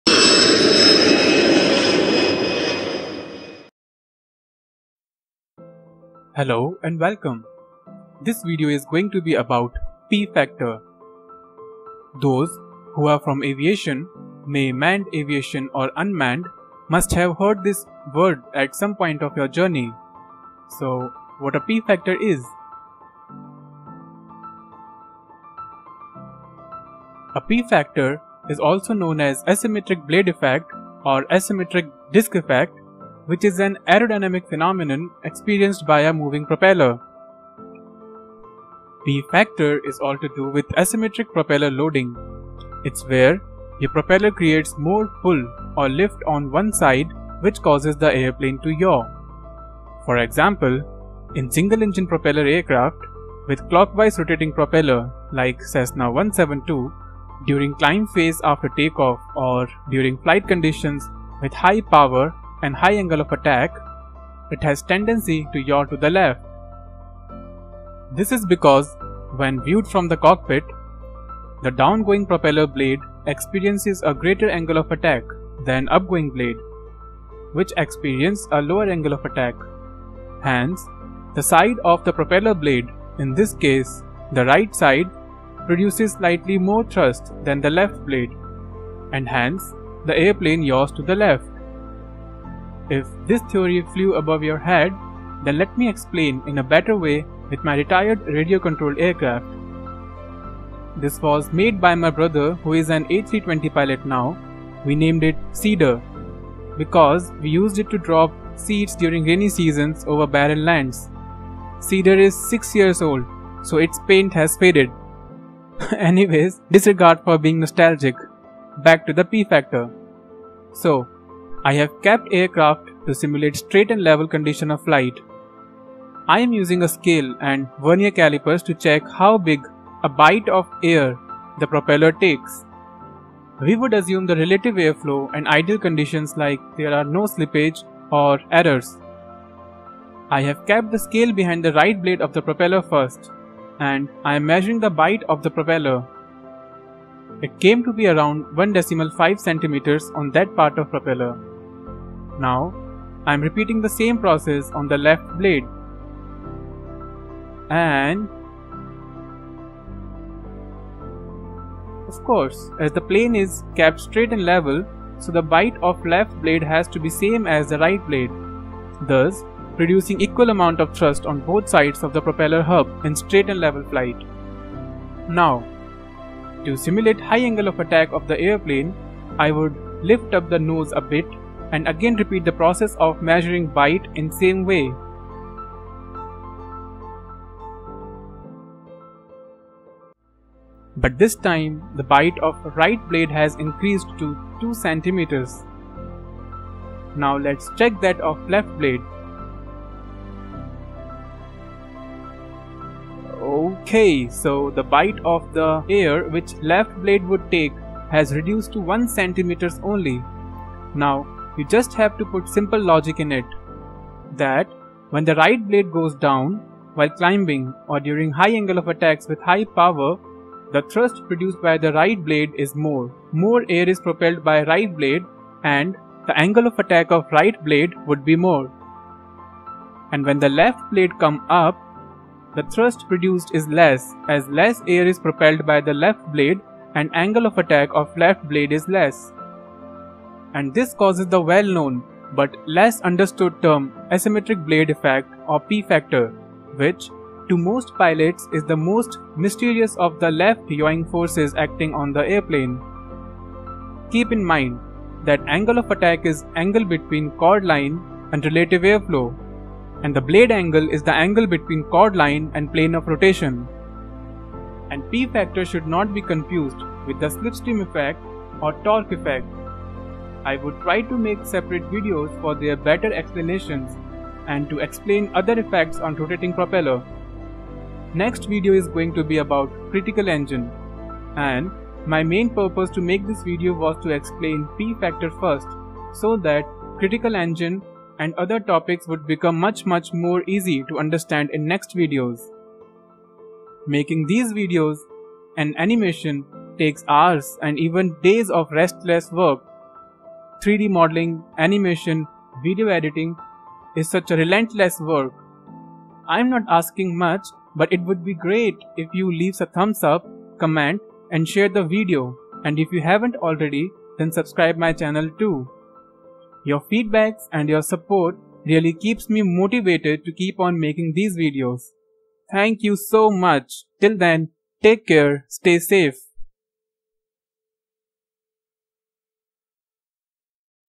P-Factor Explained !!!👨‍✈️ Technical | Propeller Aerodynamics
I`ve tried my best to explain P-Factor or Asymmetric Blade Effect acting on a rotating Propeller in simplest way.
Next Video after this would be CRITICAL ENGINE.